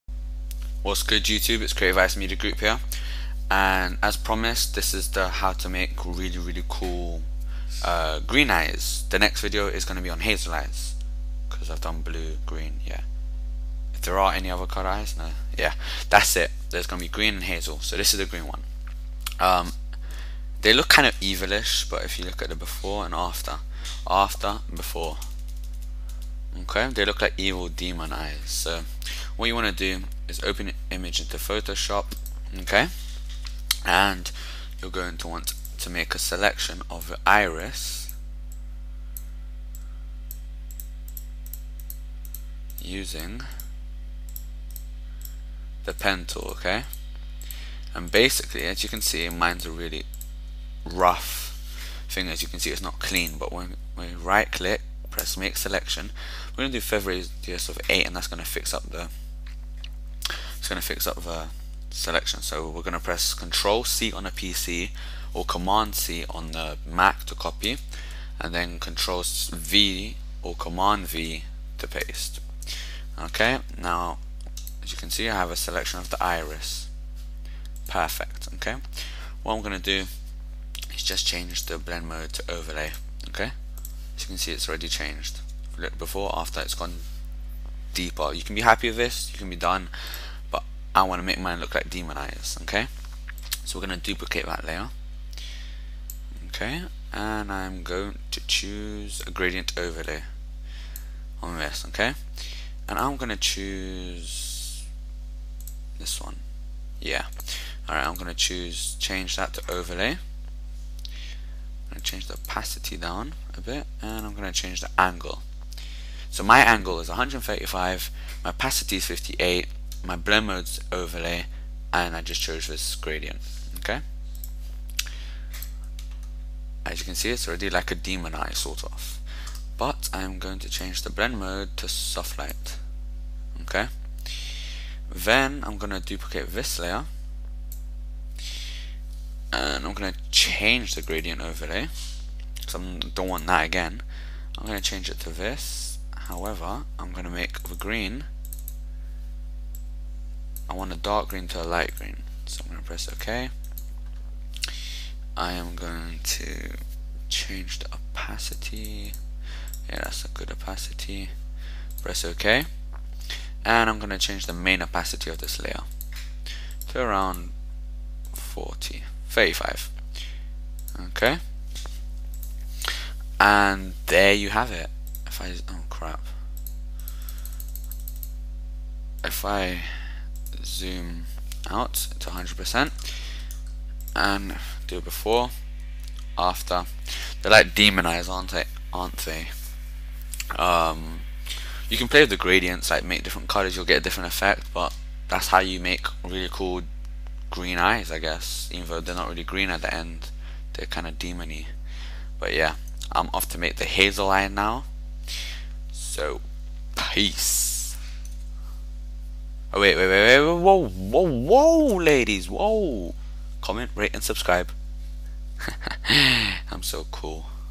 Photoshop Tutorial : How to make cool green eyes
Facebook me : search : deivid colkevicius
Ok in this tutorial i show you how to make really cool evil green eyes. In the tutorial i am using a mac and photoshop cs4